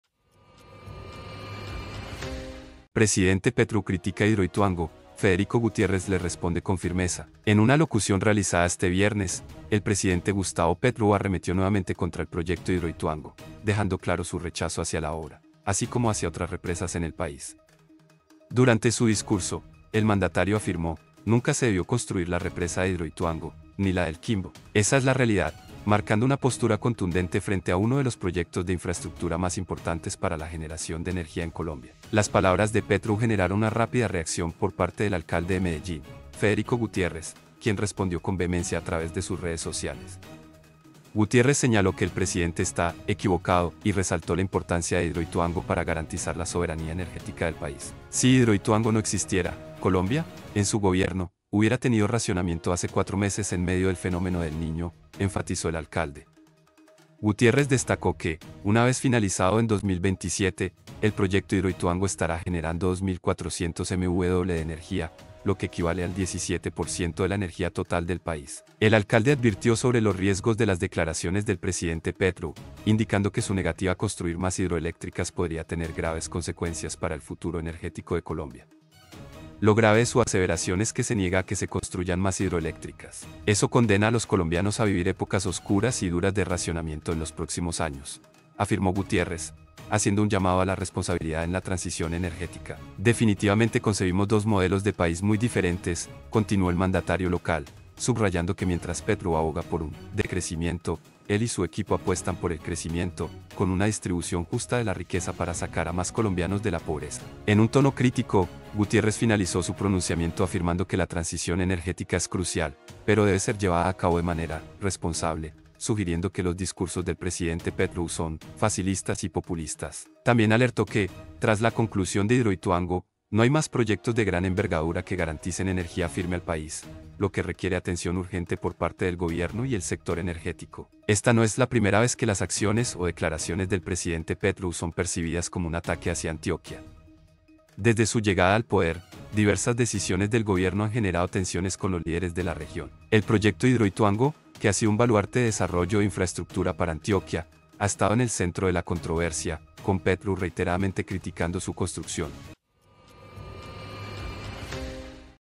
🔴 FICO RESPONDE CON FIRMESA CRÍTICAS DE PETRO A HIDROITUANGO
En una alocución realizada este viernes, el presidente Gustavo Petro arremetió nuevamente contra el proyecto Hidroituango, dejando claro su rechazo hacia la obra, así como hacia otras represas en el país. Durante su discurso, el mandatario afirmó: “Nunca se debió construir la represa de Hidroituango, ni la del Quimbo. Esa es la realidad”, marcando una postura contundente frente a uno de los proyectos de infraestructura más importantes para la generación de energía en Colombia.
IFM NOTICIAS